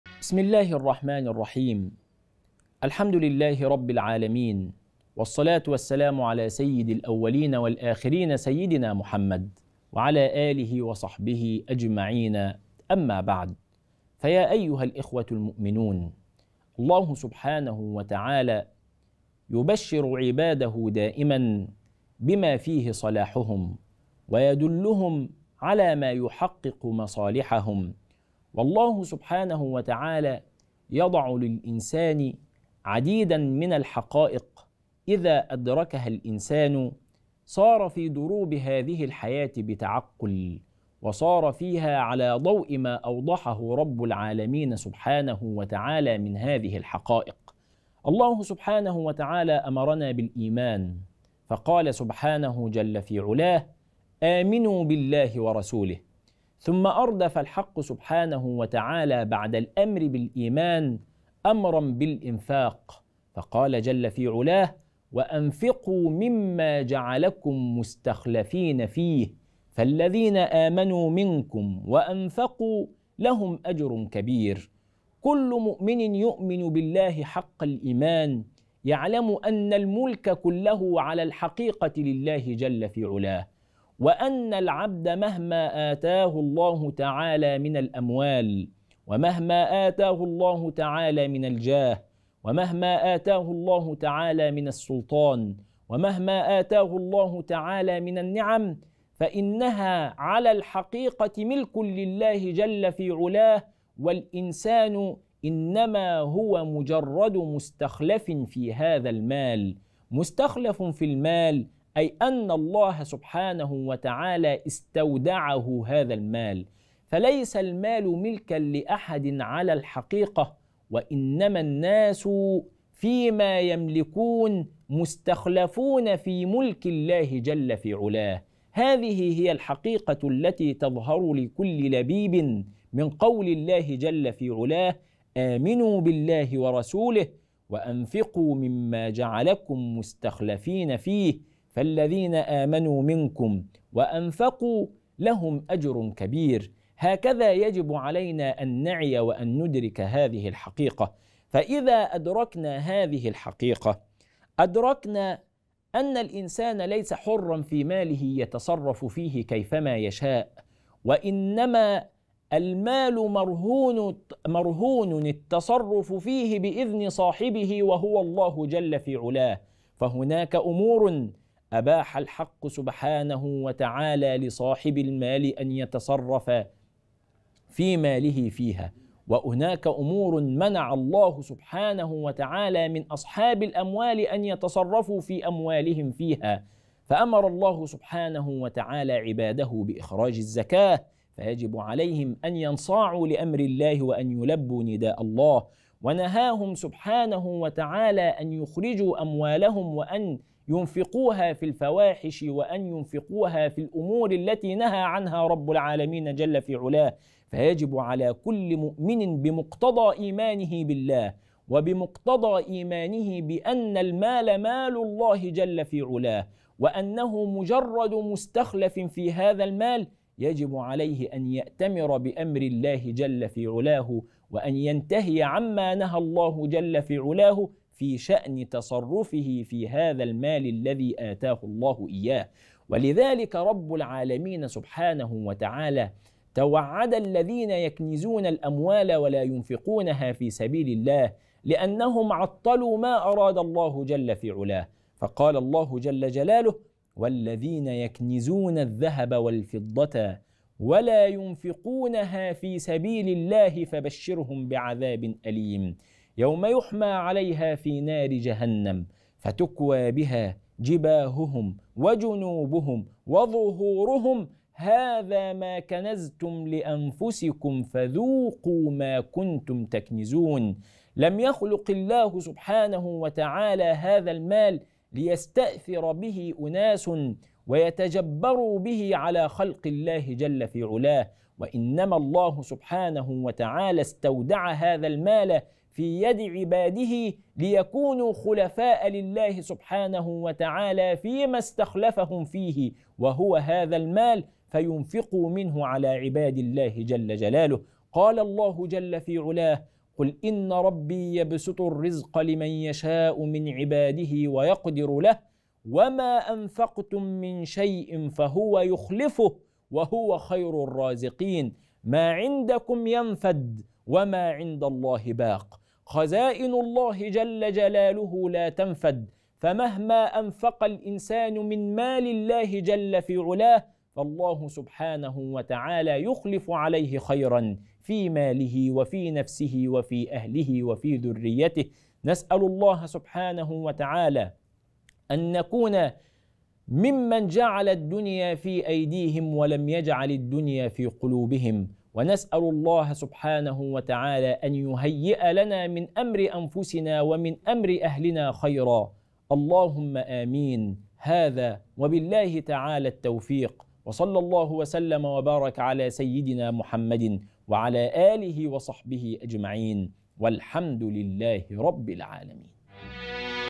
وأنفقوا ما جعلكم مستخلفين فيه مع الشيخ أحمد إبراهيم المشد عضو المركز الأزهر العالمي للفتوى
وأنفقوا ما جعلكم مستخلفين فيه مع الشيخ أحمد إبراهيم المشد عضو المركز الأزهر العالمي للفتوى الإلكترونية